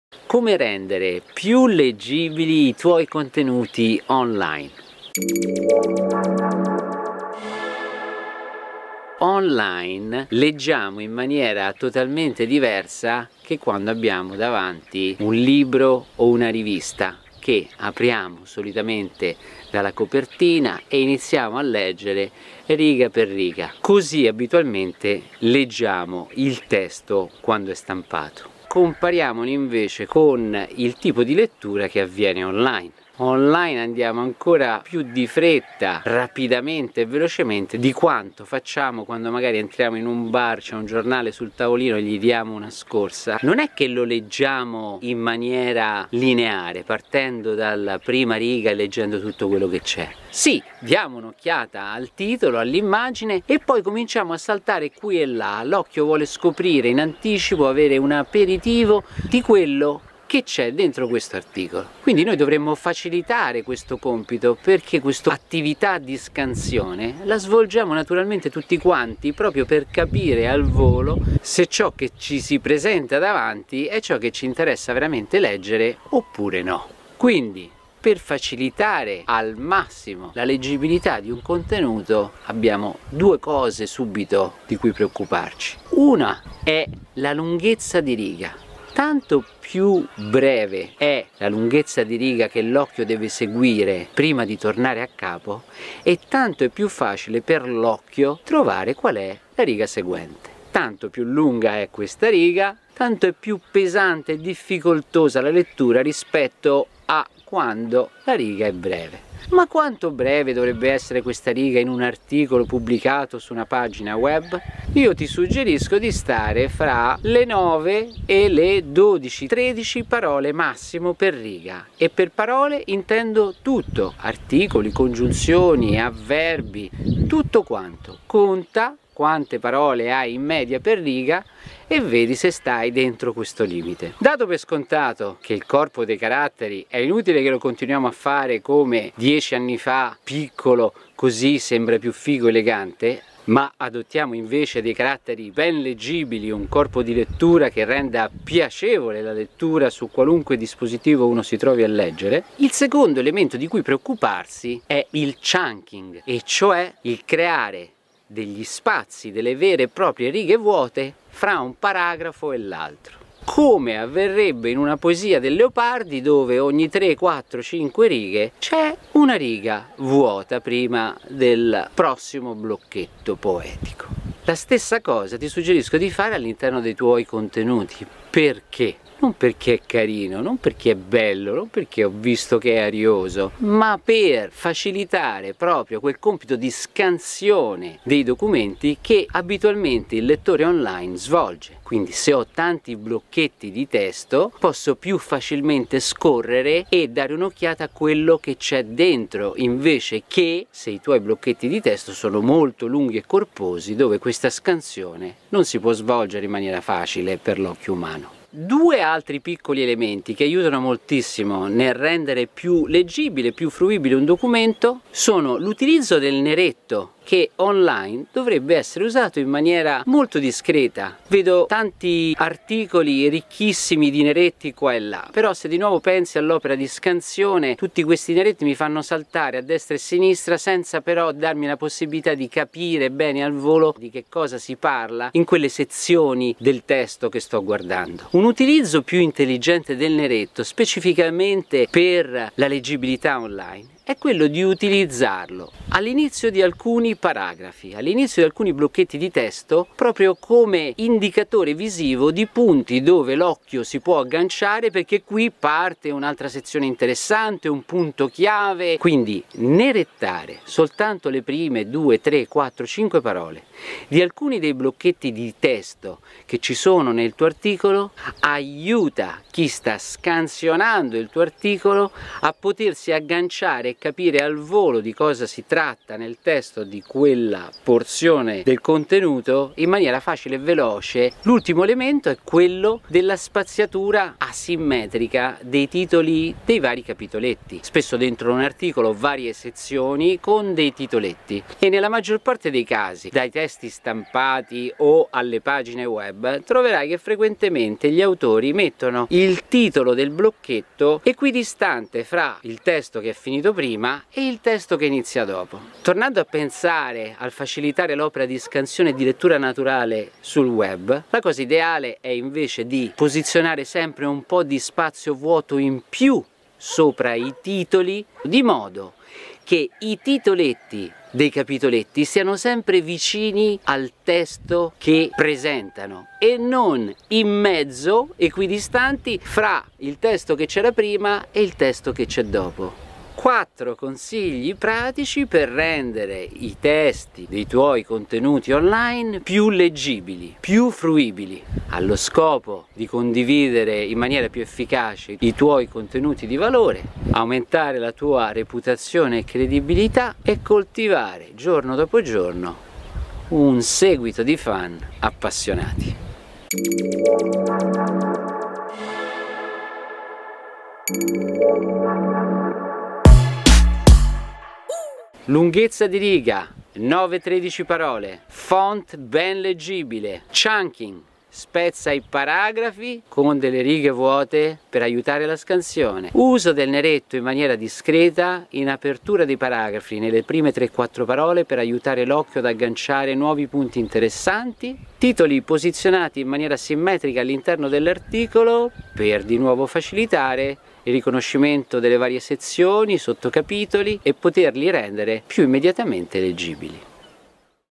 Come rendere più leggibili i contenuti online
Leggibilità online: quattro consigli pratici per migliorare la fruibilità dei tuoi contenuti online.
Risorse:

1. Riga corta - 9-12 parole per riga

3. Uso del neretto

4. Spaziatura asimmetrica dei titoli delle sotto sezioni

Lettura consigliata: "Dynamics in Document Design" by Karen Schriver